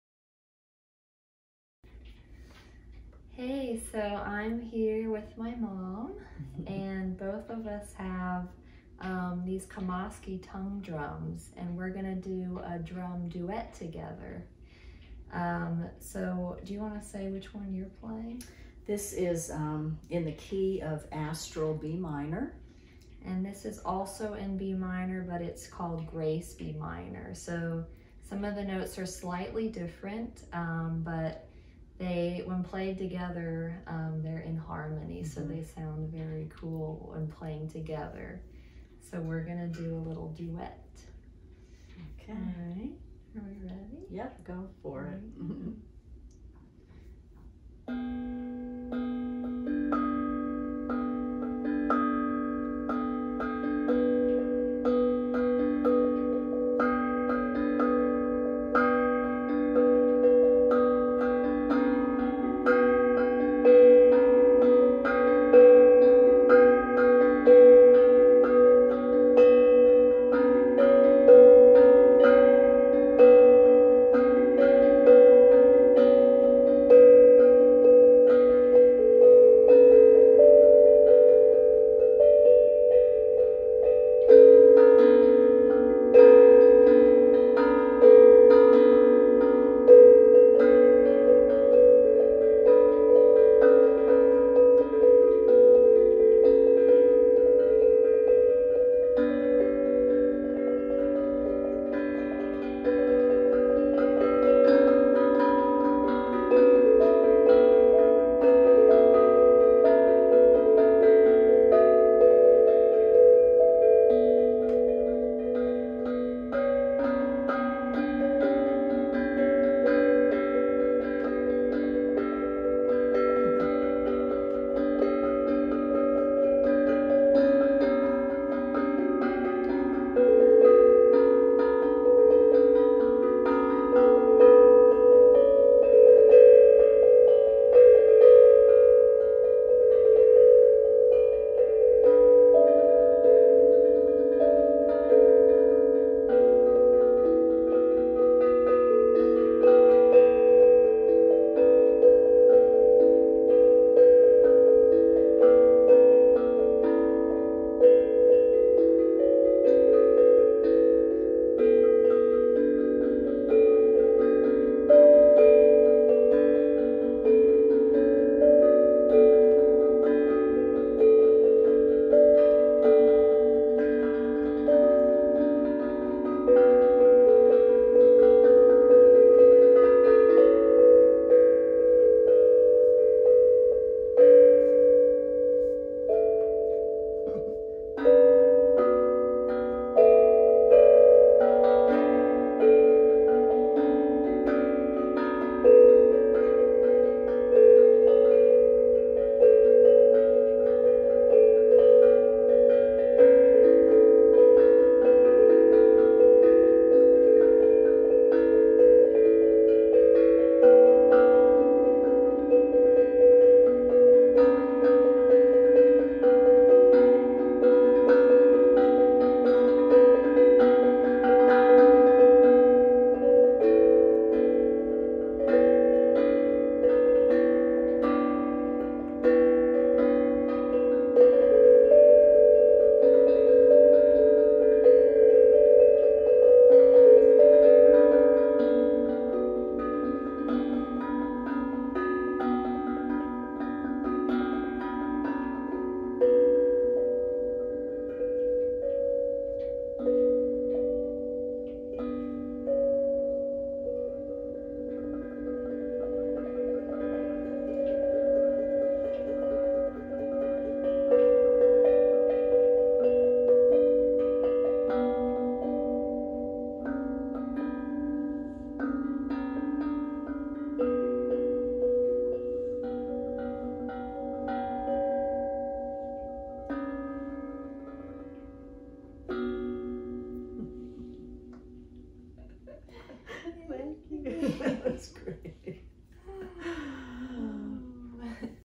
Tongue Drumming- A Mother Daughter Duet
My mom and I decided to play together with our tongue drums and create a duet! I'm playing with the Grace B-Minor scale and my mom is playing with the Astral B-Minor scale. When played together they create a beautiful harmony.

My mom and I love drumming together and often do joint workshops.

To learn more about my moms drum workshops visit her

To learn more about my drum workshops visit my